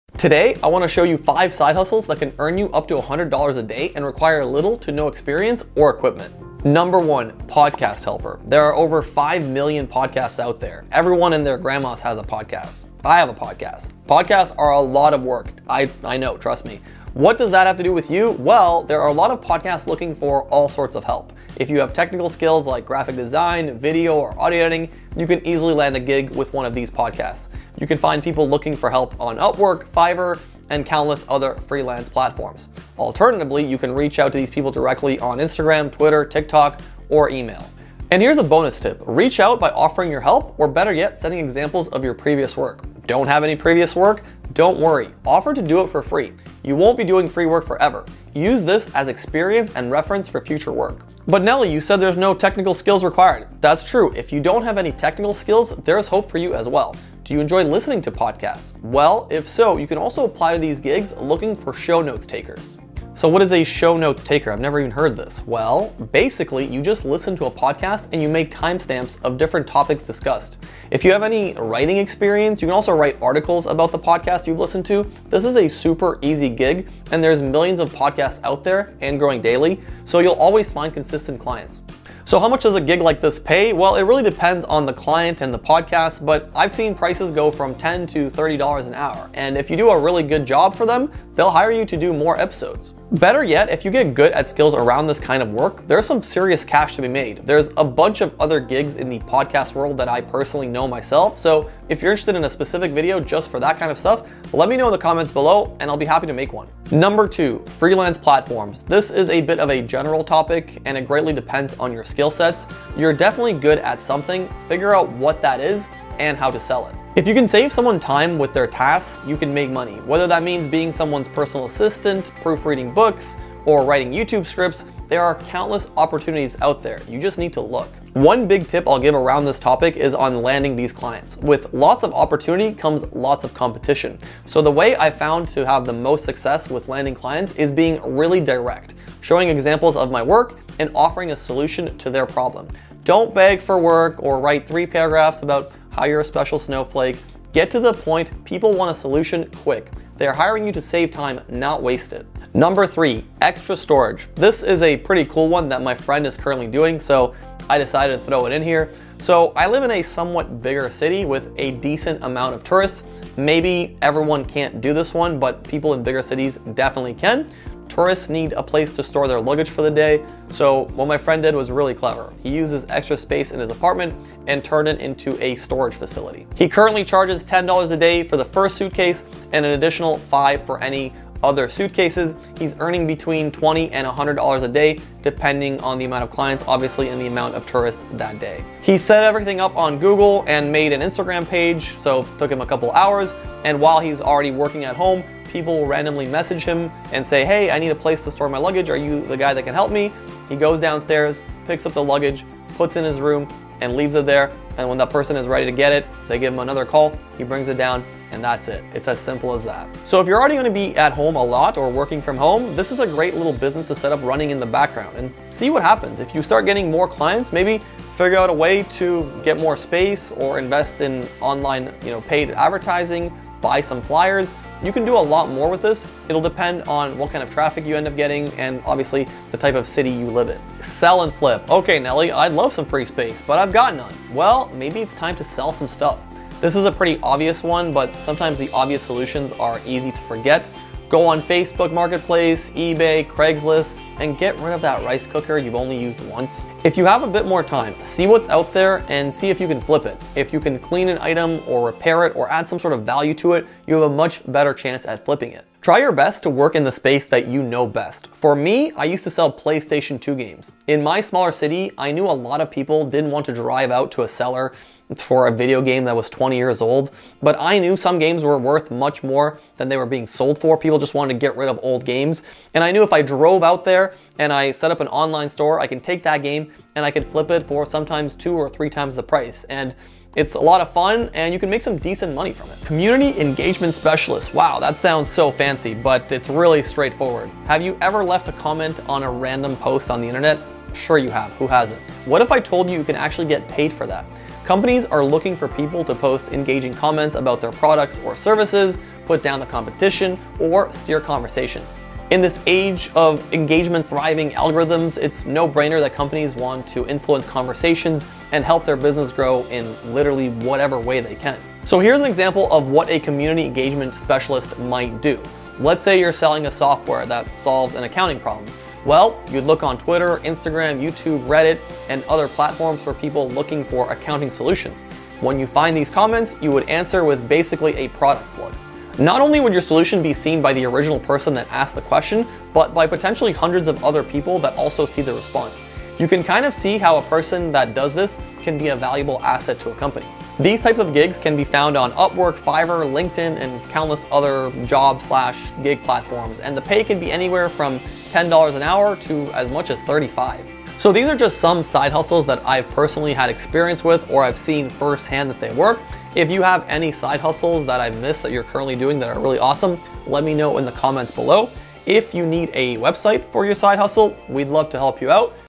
Side Hustles I Wish I Knew About!
Everyone loves making some cash on the side. Today we'll be going over some different side hustles I've personally done myself or have seen first hand. Most of these don't require much skill or technical expertise so you can start them right away.

Have a business? You need a website! We'll build it for you in less than 7 days

0:00 Podcast Helper
1:48 Freelance Gigs
2:32 Renting Out Storage Space
3:50 Sell & Flip (Work within your space)
4:43 Community Engagement Specialist